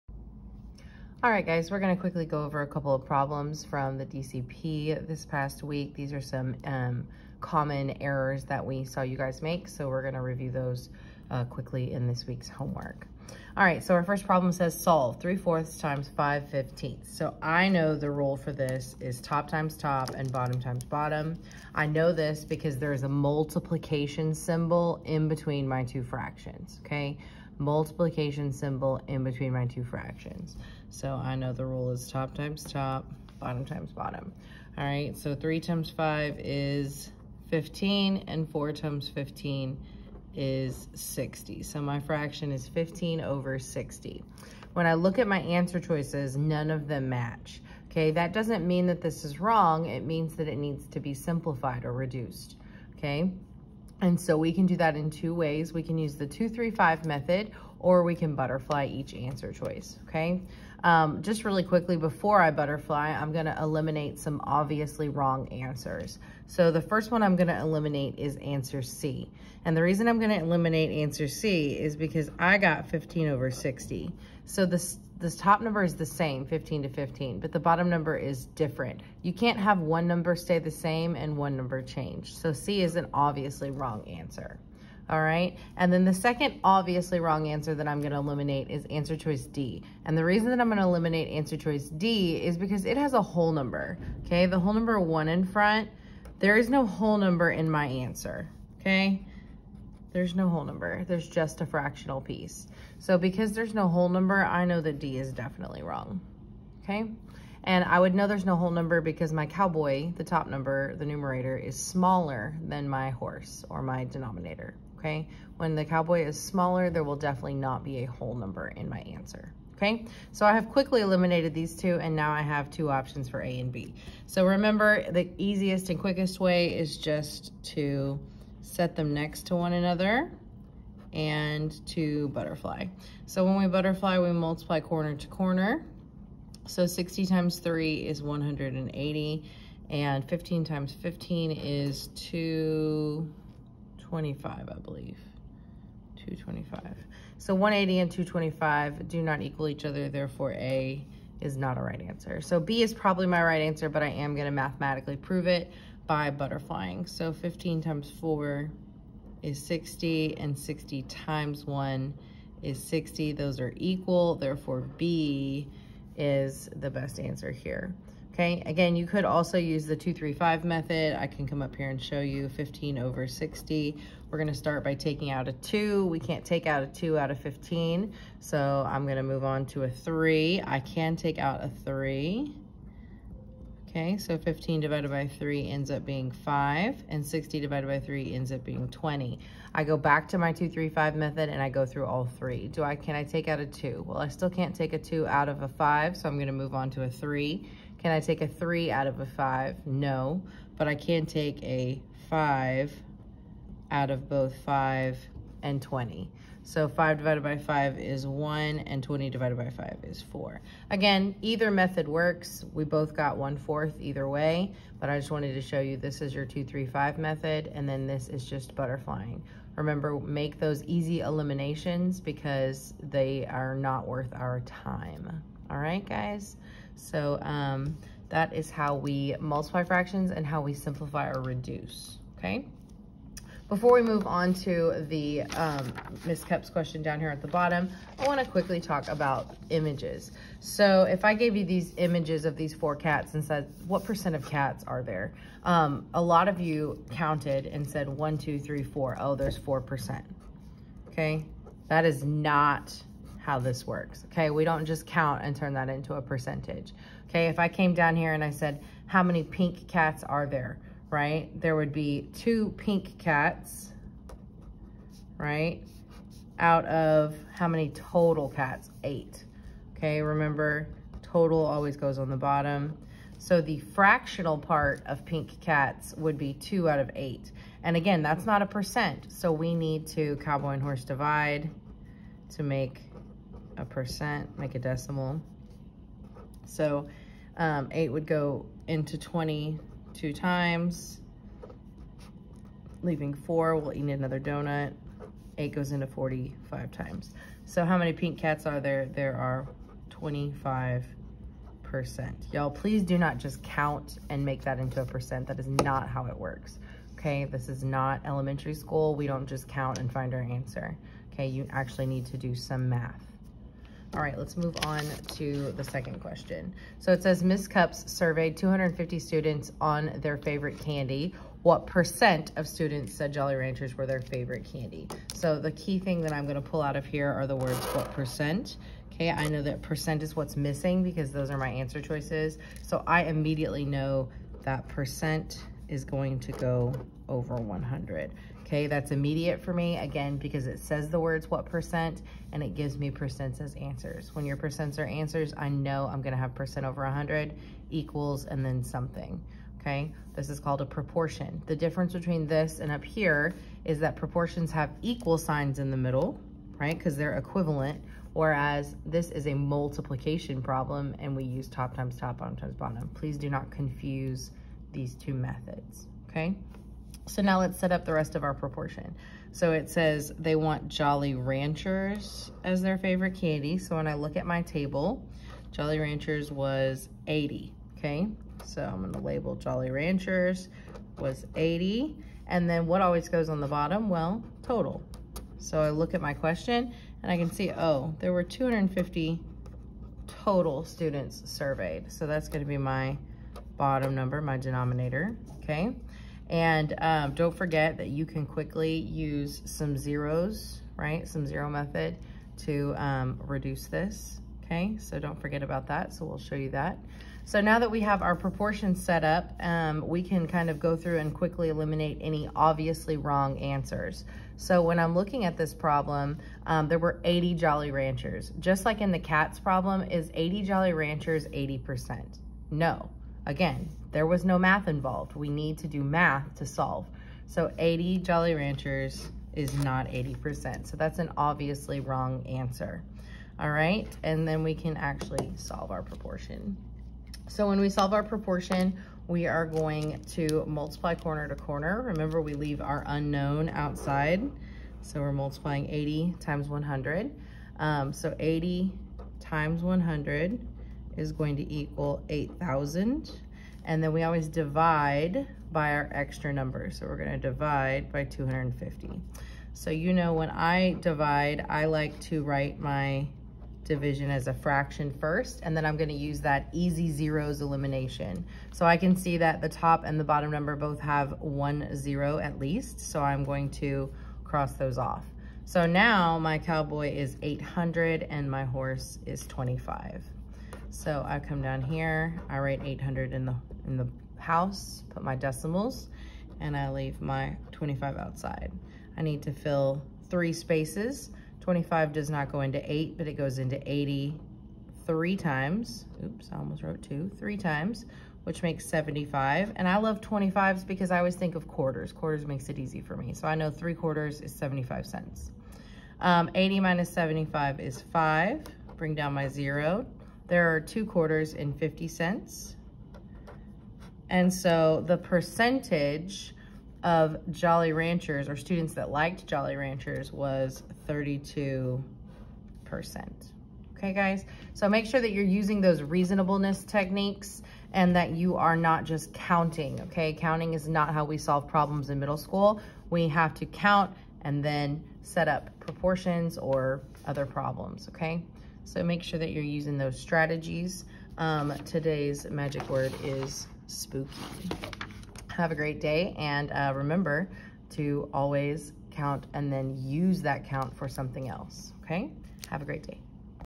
Fractions & Percent Proportions Examples
6.3E & 6.5B 6th Grade STAAR Examples
Multiplying Fractions with Simplifying
Set Up and Solve Percent Proportions from a table